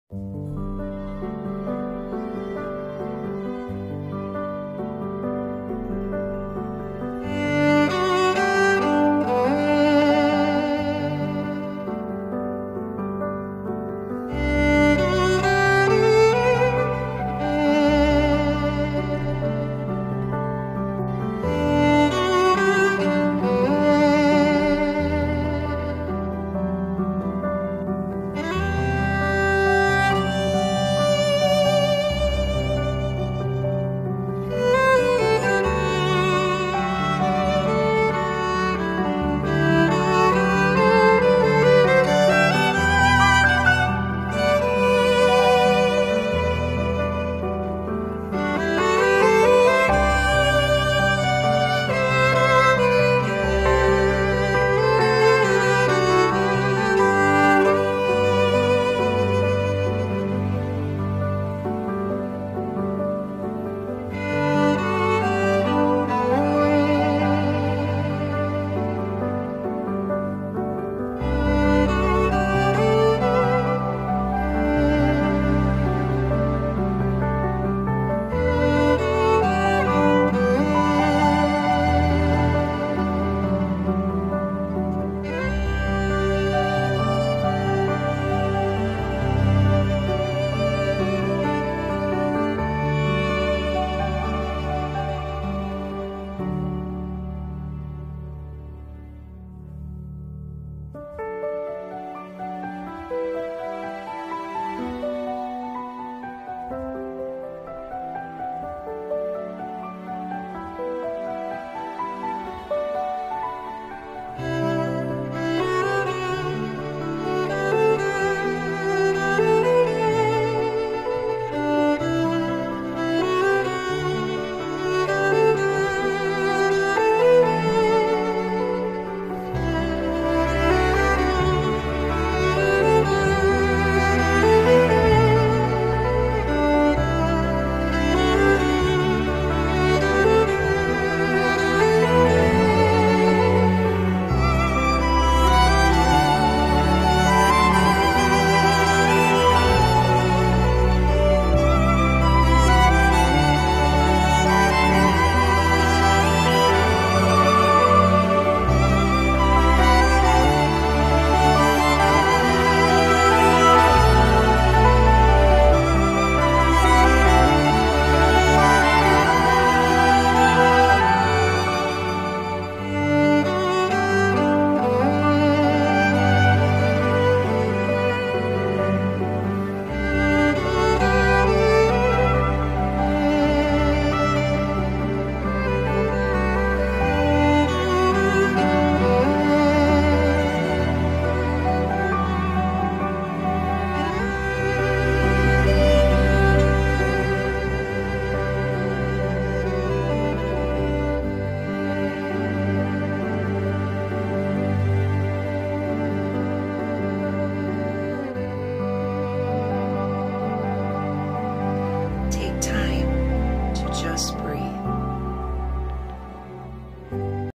Take Time To Just Breathe...
Hectic day? Take a few minutes to just breathe...